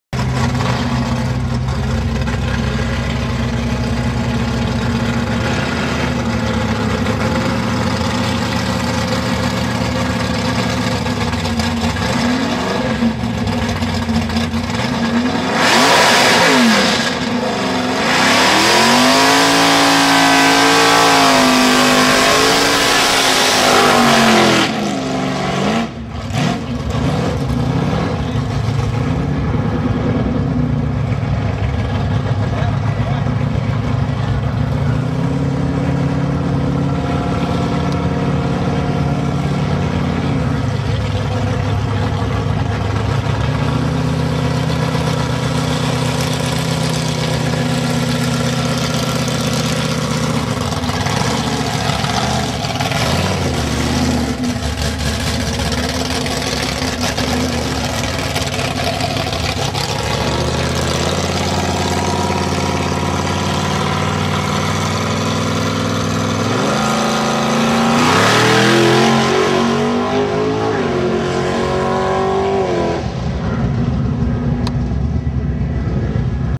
Harry Potter??????????
Ford Anglia with Aussie Holden Muscle under the bonnet